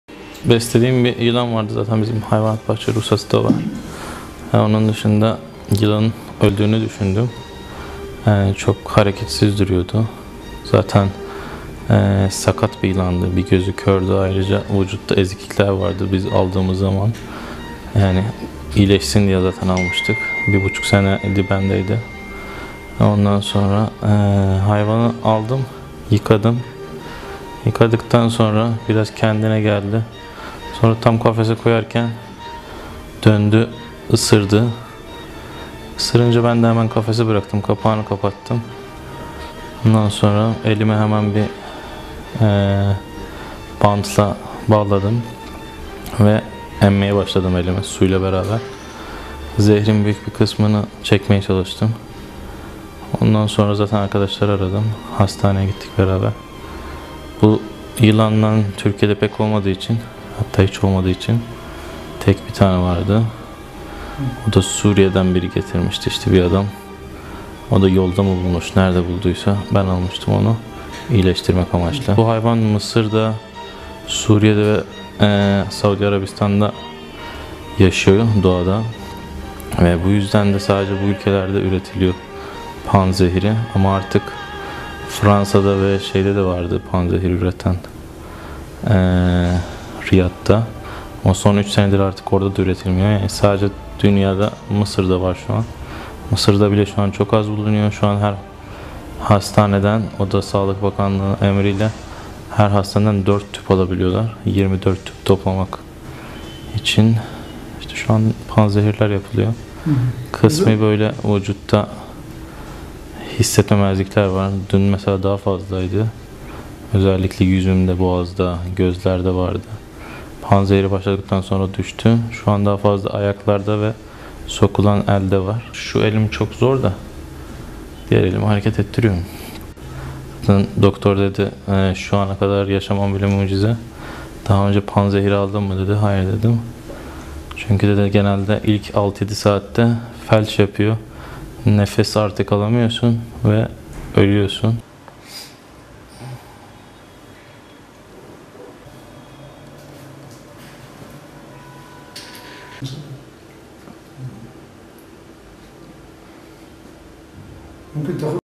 AREF GHAFOURİ ÖLECEK Mİ ?
ÜNLÜ SİHİRBAZ AREF GHAFOURİ YILAN'IN ISIRDIĞI ANI ANLATTI !
PANZEHİR İSE YALNIZCA MISIR'DA BULUNUYOR !